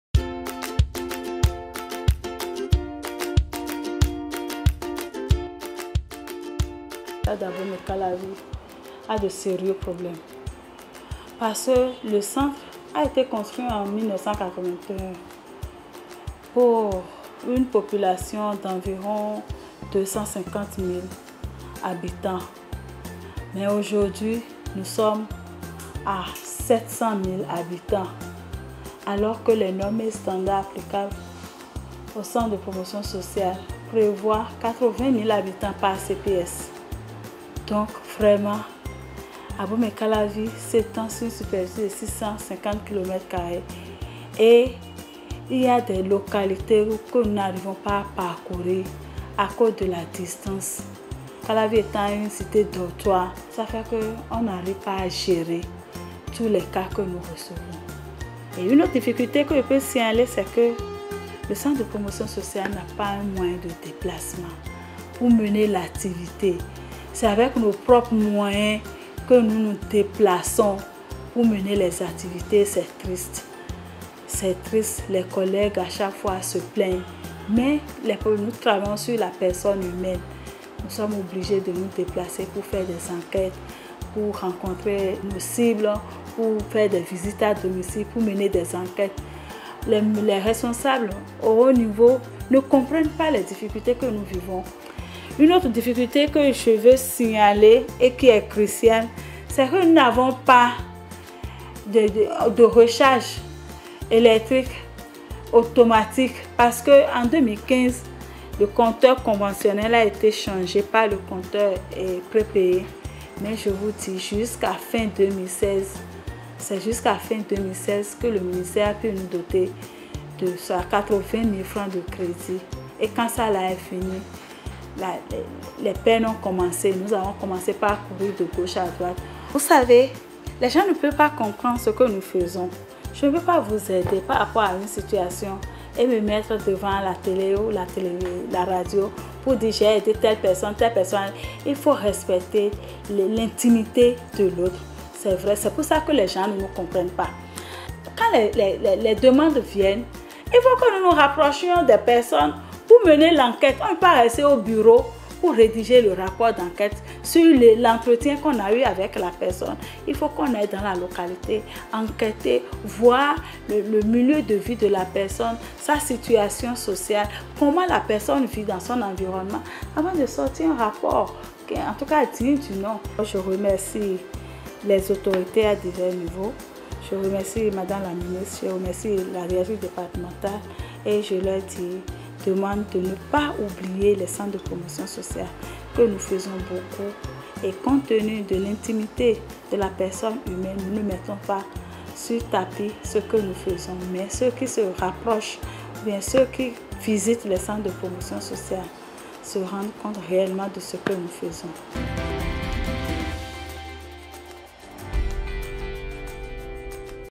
Bénin : Tata Masouratou Katchon expose les difficultés du fonctionnement du CPS d’Abomey-Calavi
Les centres de promotion sociale vivent, pour la plupart, dans un état peu confortable pour les agents qui y travaillent. Des conditions de travail peu reluisantes, du manque du minimum pour le fonctionnement de ces centres et surtout l’immensité des dossiers de ces cas sociaux à gérer font que les acteurs sont obligés de lancer l’alerte afin que les dirigeants entendent leur cri et voler à leur secours. A l’instar des autres centres du pays, le centre de promotion sociale d’Abomey-Calavi vit également le même calvaire. Dans cet entretien, la responsable de ce centre, Tata Massouratou Katchon, nous expose les difficultés que vivent ses collaborateurs dans l’accomplissement de leurs tâches professionnelles.